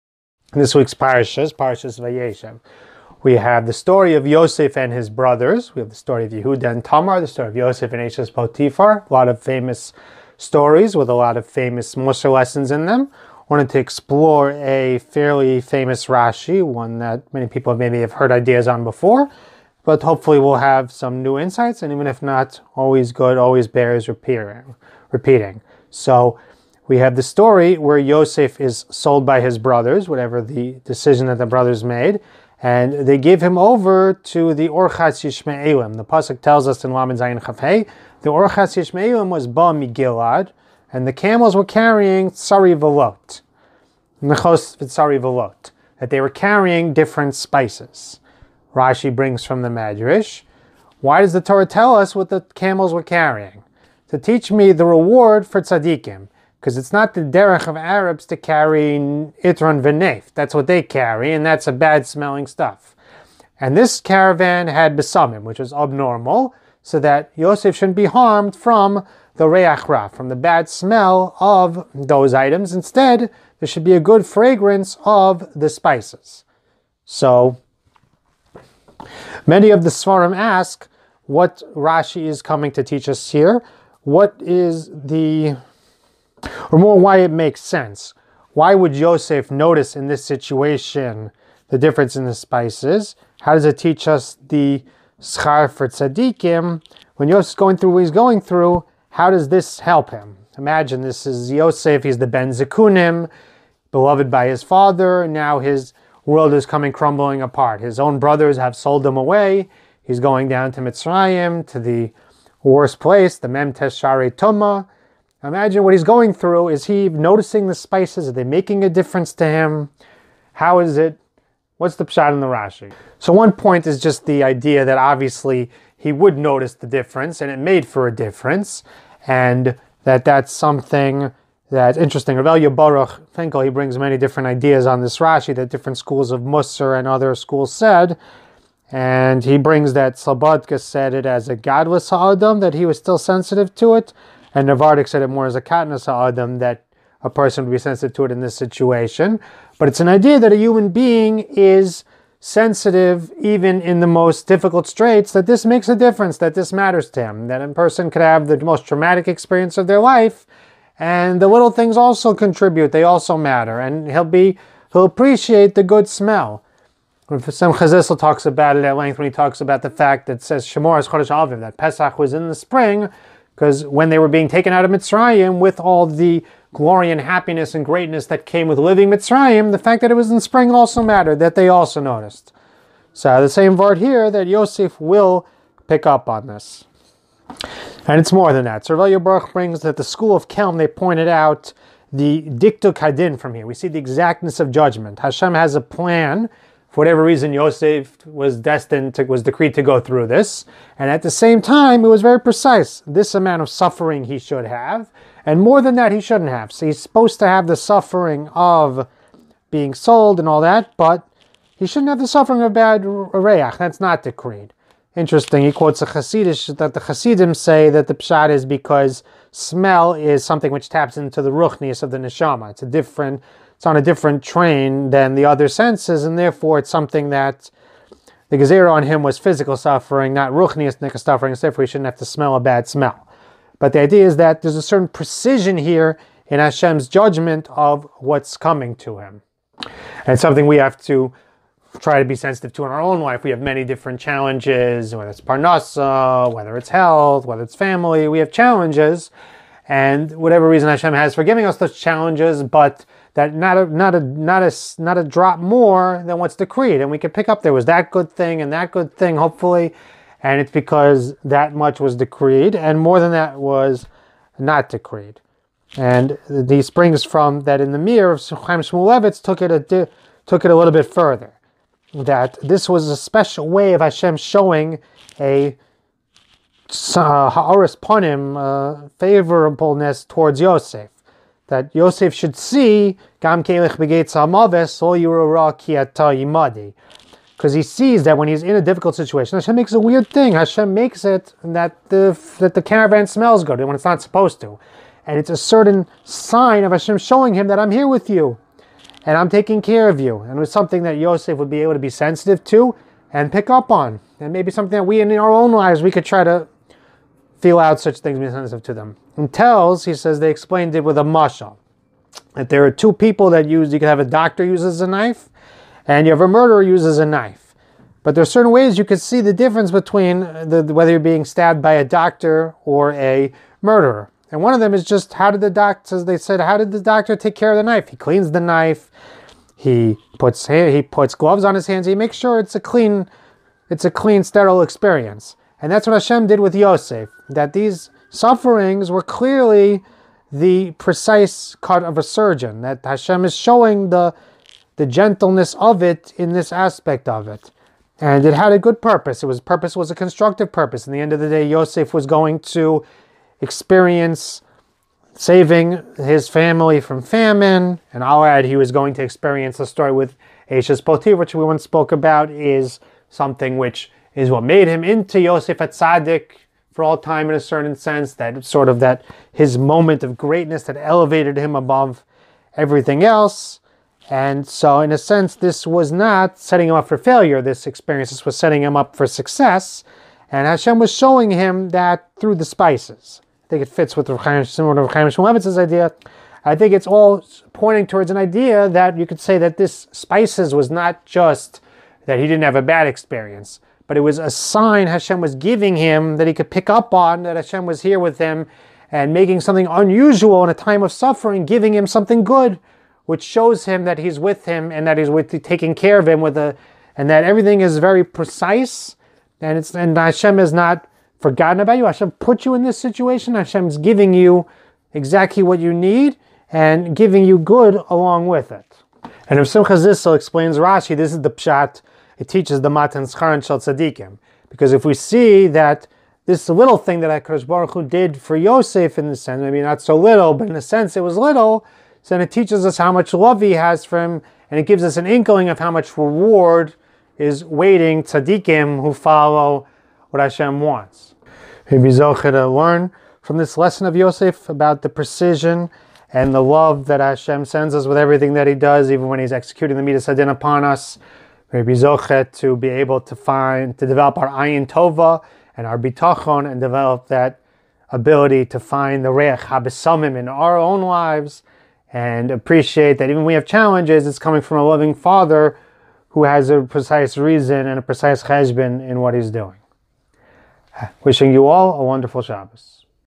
Vayeishev - The Good Spices in Life
Rashi quotes Chazal teach us that Yosef was taken in a caravan filled with good spices. How did that help him in this difficult situation? A few different lessons the sefarim take from this Rashi.